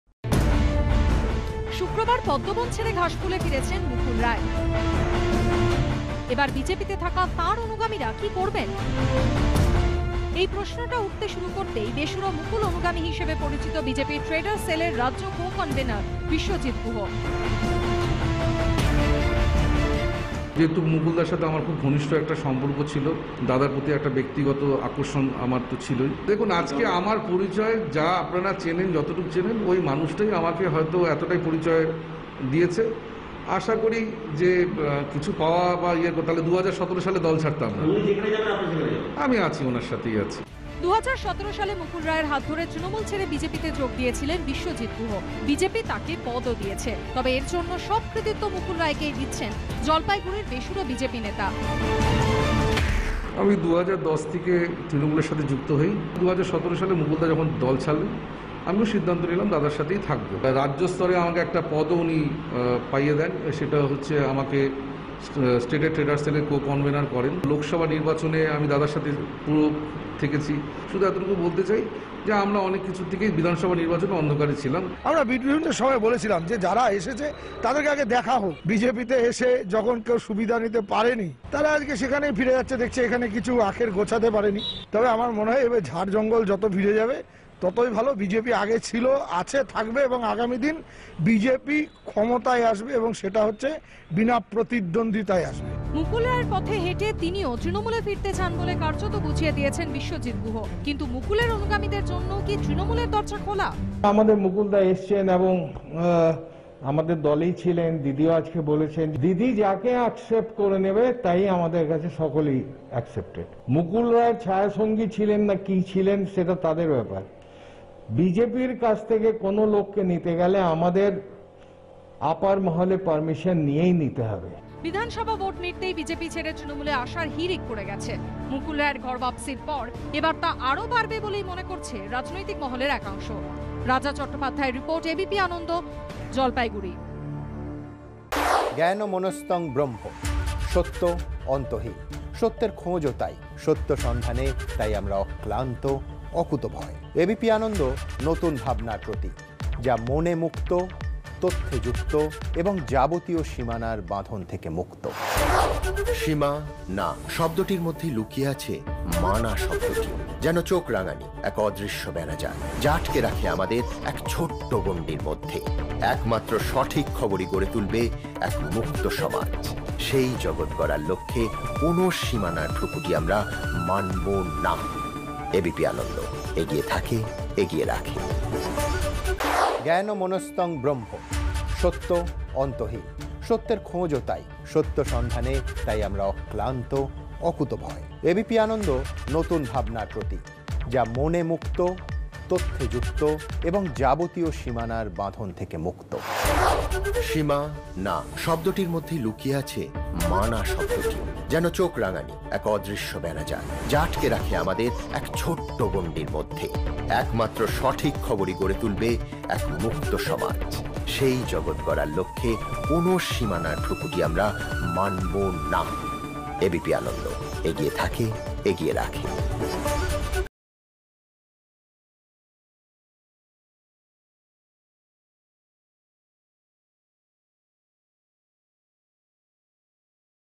WB Politics: এবার জলপাইগুড়িতে পদ্মশিবিরে ভাঙন? জল্পনা বাড়ালেন মুকুল-অনুগামী BJP নেতা বিশ্বজিৎ গুহ
মুকুল রায় (Mukul Roy) যেখানে যাবেন, ওনার সঙ্গে আমিও থাকব। মুকুল তৃণমূলে (TMC) ফিরতেই বেসুরো তাঁর অনুগামী হিসেবে পরিচিত জলপাইগুড়ির (Jalpaiguri) বিজেপি (BJP) নেতা বিশ্বজিৎ গুহ (Biswajit Guha)। তাঁকে দলে ফেরানো হবে কি না সিদ্ধান্ত নেবে শীর্ষ নেতৃত্ব, জানালেন জেলা তৃণমূলের সভাপতি। শুক্রবার ঘাসফুলে ফিরেছেন মুকুল রায়। এবার বিজেপিতে থাকা তাঁর অনুগামীরা কী করবেন?